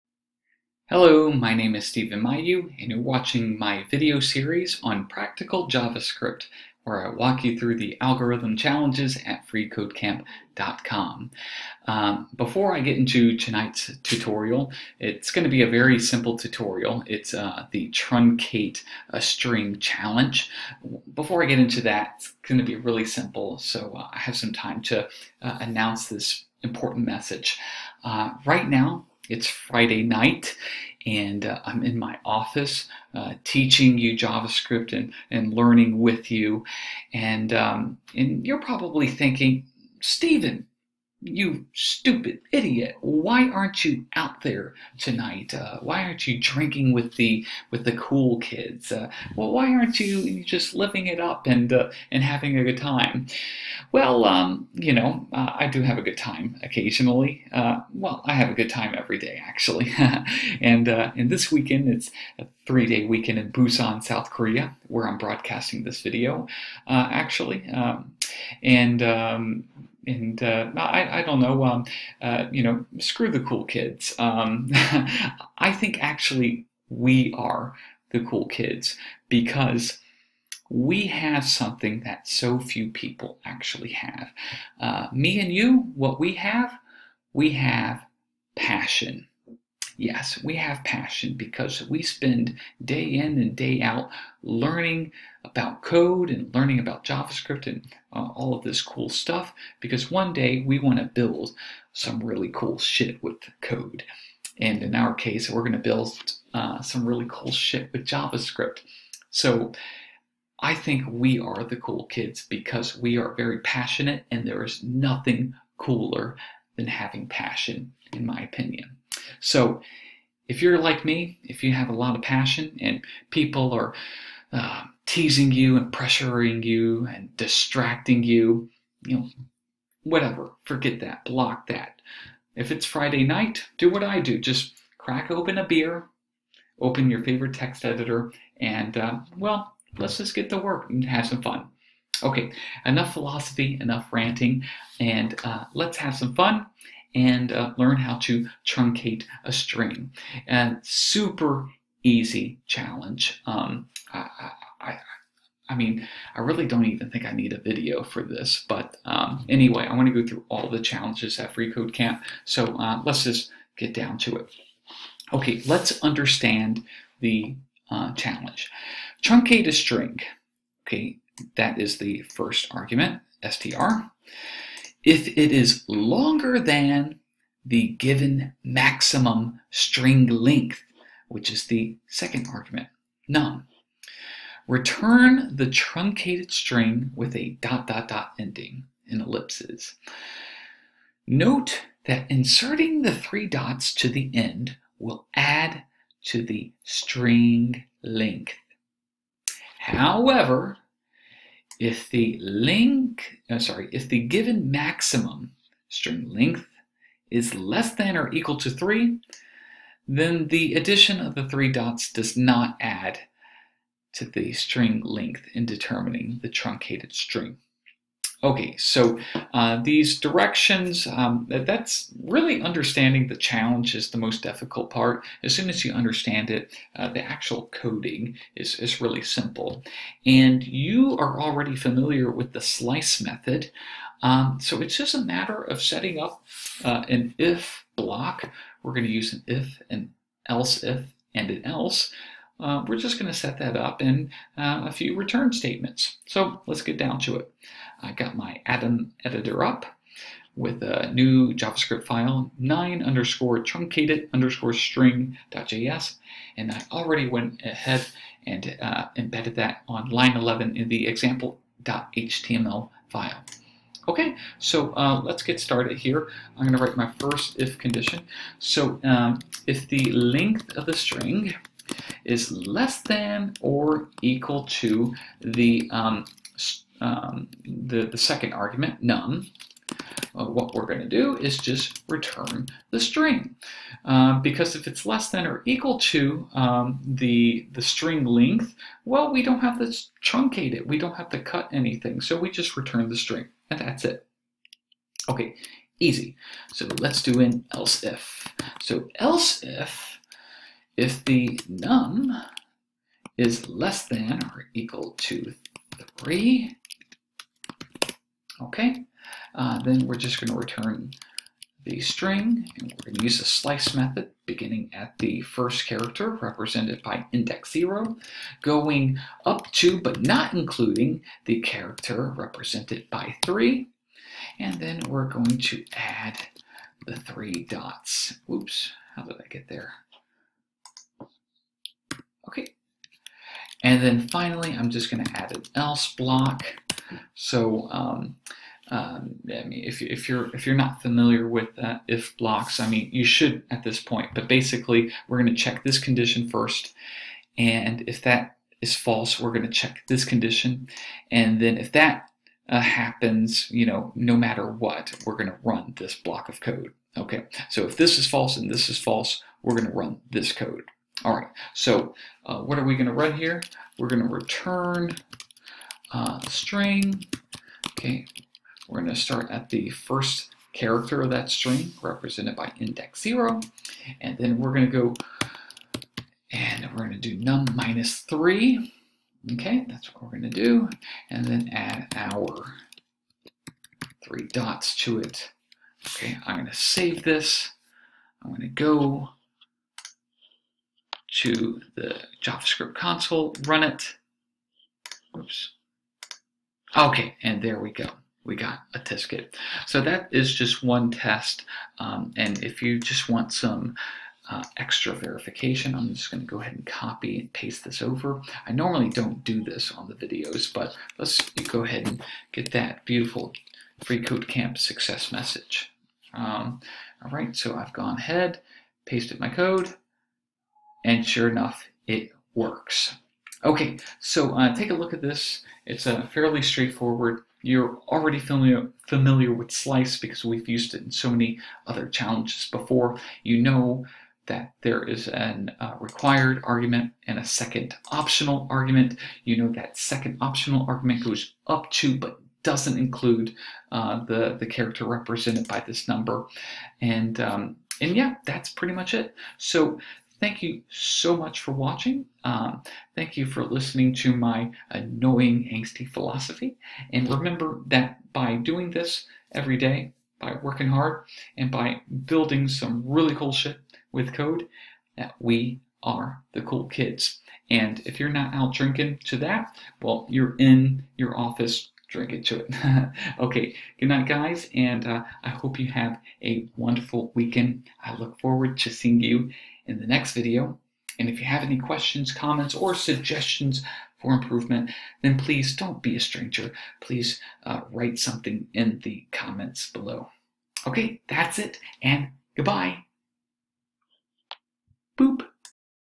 Truncate a String in JavaScript
In this video series, Stephen Mayeux (stephenmayeux.com) walks through the algorithmic challenges at Free Code Camp.

Nothing new here! Just a simple `.slice()` method with an `if`/`else if`/`else` block.